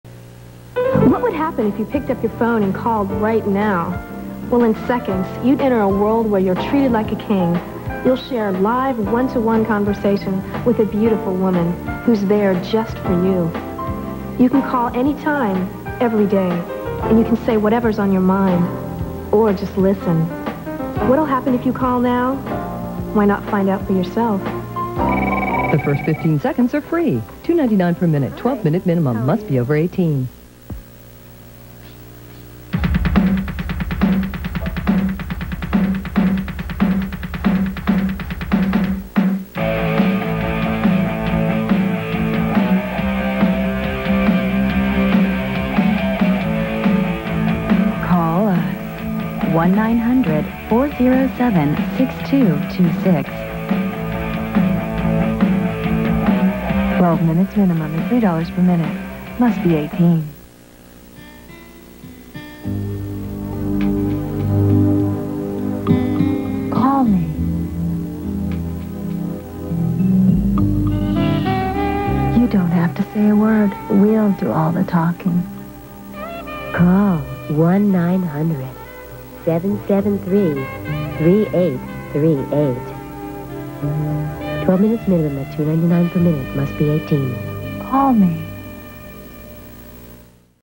The First 15 Seconds Are FREE | 900-Number TV Commercials from the 1990s
Three more 900-number television commercials from the early 1990s. These 900 and 976 lines were extremely popular and the competition for late-night ad slots was fierce. Many of the actors/actresses that appeared in 900-number commercials were adult film stars.

I offer these as contrast to our 900-number commercials, and to show how our competitors were advertising their 900-number services.